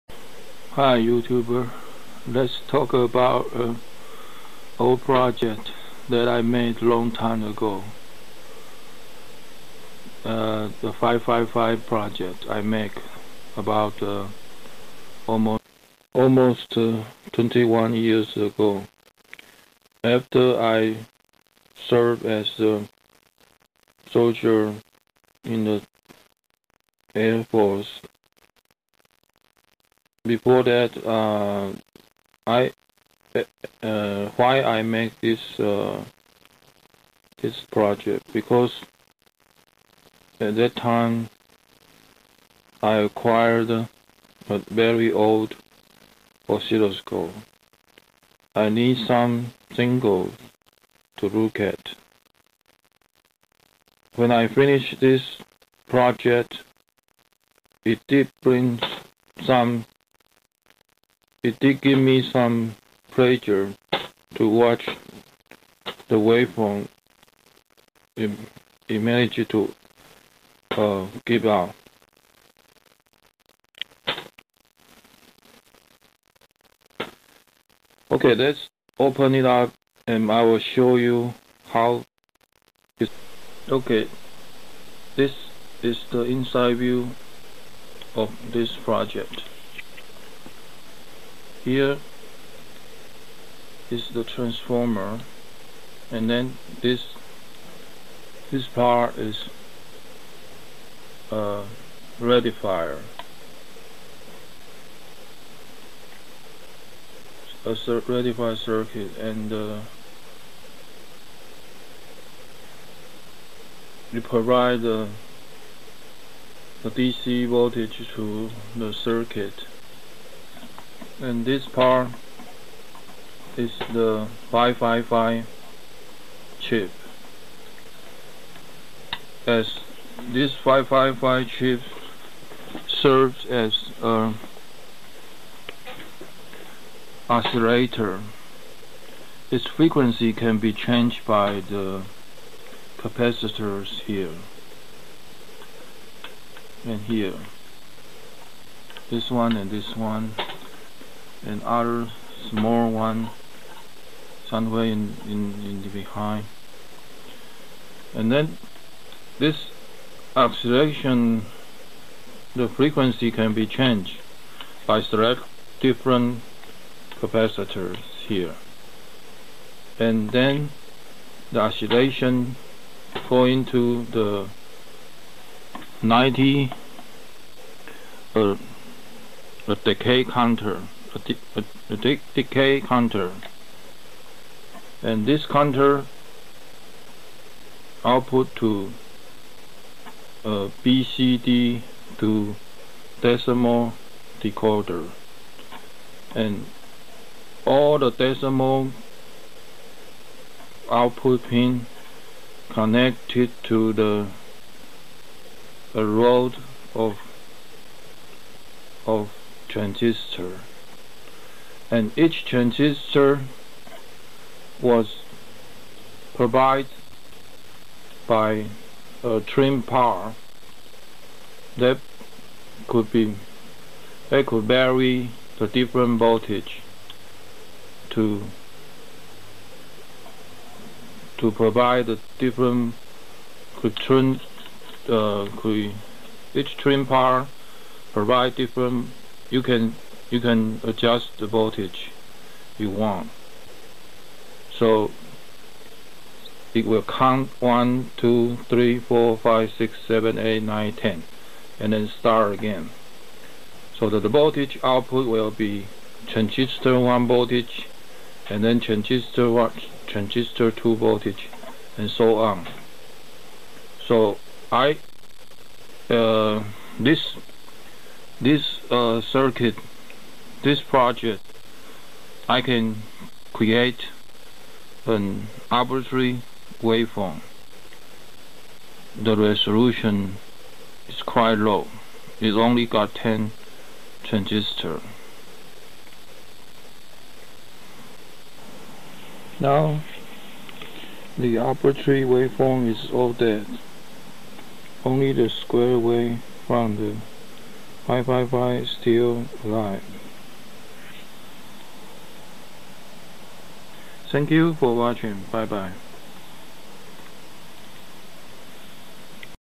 555 arb waveform project
a project i did long time ago, 21 yrs old.parts using is 555,74LS44,74LS90,and some transistor,trim pot,resistor,capacitor.it serve a goal to make waveform for an old oscilloscope to look at.it did provide me some joy.now arb is dead,only square alive.it need a output buffer,maybe that is another project sometime in future.don't know if money allow me XD. nice memory but sad story.:(